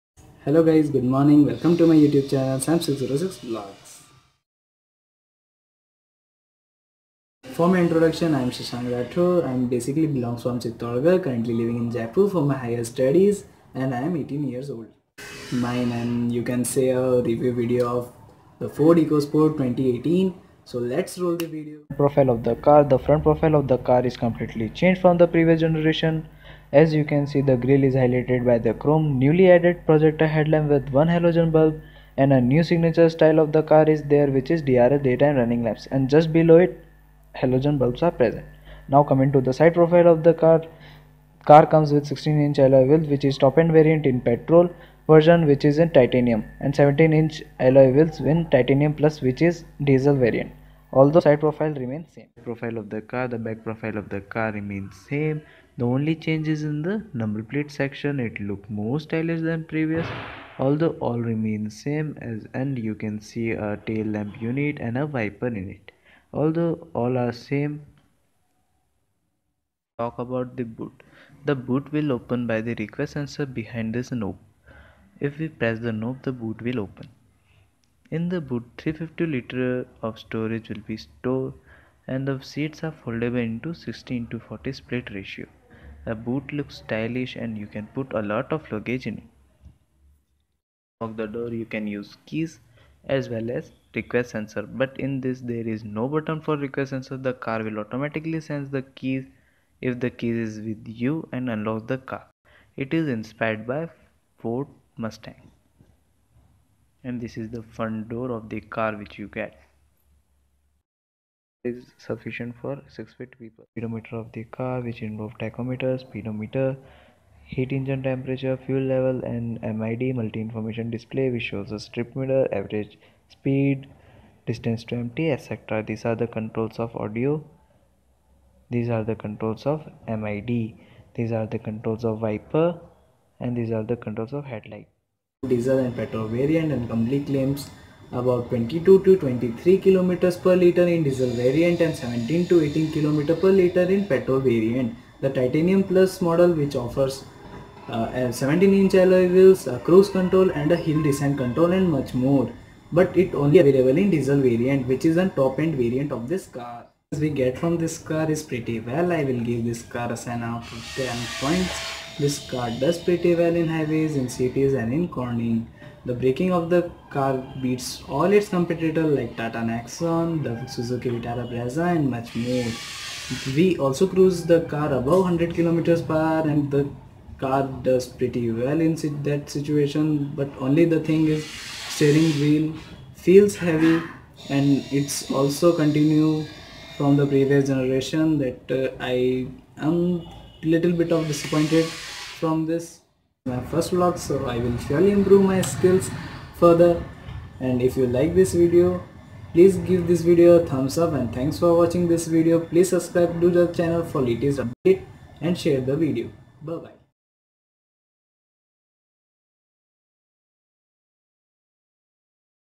Ford EcoSport 2018 || First Vlog || speedometer || Review || Test Ride Experiance.
Engine Displacement(cc) 1497
Maximum Power 121.36bhp@6500rpm
Maximum Torque 150Nm@4500rpm
Engine Description 1.5 litre 121.36bhp Petrol Engine
Turning Radius 5.3 metres
Turbo Charger No
Super Charger No
Fuel Supply System Direct Injection
Gear box 5 Speed
Steering Gear Type Rack & Pinion
CAPACITY
Seating Capacity 5
No of Doors 4
Length 3998mm
Width 1765mm
Height 1647mm
Ground Clearance 200mm
Ground Clearance Unladen -
Wheel Base 2519mm
Front Tread -
Rear Tread -
Kerb Weight 1242Kg
Gross Weight 1660Kg
Front Headroom -
Front Legroom -
Rear Headroom -
Rear Legroom -
Fuel Tank Capacity (Liters) 52
Cargo Volume 352-litres
Tyre Size 205/60 R16
Tyre Type Tubeless,Radial
Alloy Wheel Size 16 Inch